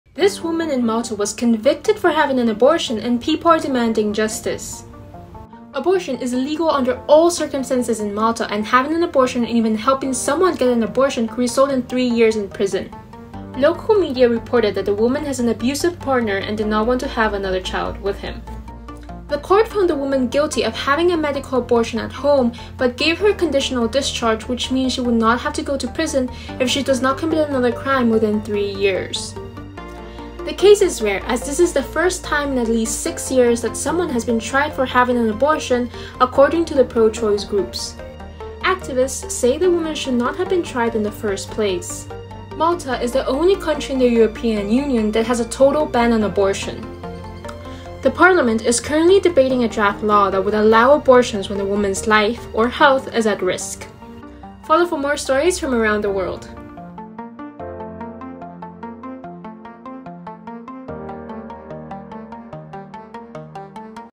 This Woman In Malta Was Convicted For Having An Abortion And People Are Demanding Justice
A woman in Malta was convicted for having an abortion and people are demanding justice.

Abortion is illegal under all circumstances in Malta, and having an abortion or helping someone to get an abortion could result in three years in prison.

Local media reported that the woman has an abusive partner and did not want to have another child with him.

The court found the woman guilty of having a medical abortion at home, but gave her a conditional discharge, which means she will not have to go to prison if she does not commit another crime within three years.

The case is rare as this is the first time in at least six years that someone has been tried for having an abortion, according to the pro-choice groups.

Activists say the woman should not have been tried in the first place.

Malta is the only country in the European Union that has a total ban on abortions.

The parliament is currently debating a draft law that would allow abortions when the woman’s life or health is at serious risk.